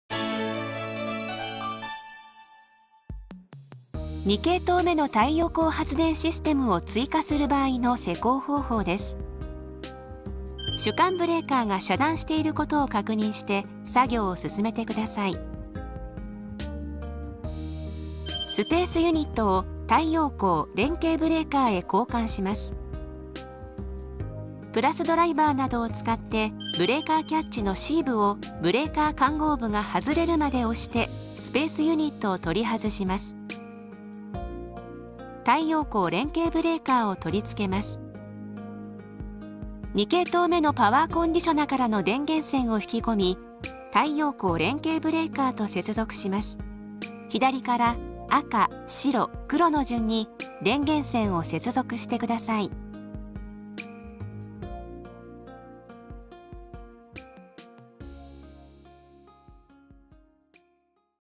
太陽光発電を追加する（２系統目）｜スマートコスモ［マルチ通信型］ | Panasonic
パナソニックのスマートHEMS

「スマートコスモ［マルチ通信型］」で２系統目の太陽光発電システムを追加する場合の施工方法です。

変更したあとは、必ず【回路設定】を行ってください。
【回路設定】の施工動画はこちら

■スマートコスモ［マルチ通信型］の施工・設定方法　動画メニュー

■AiSEG2（7型モニター機能付）［MKN713］の施工・設定方法　動画メニュー

■AiSEG2 ［MKN704］の施工・設定方法　動画メニュー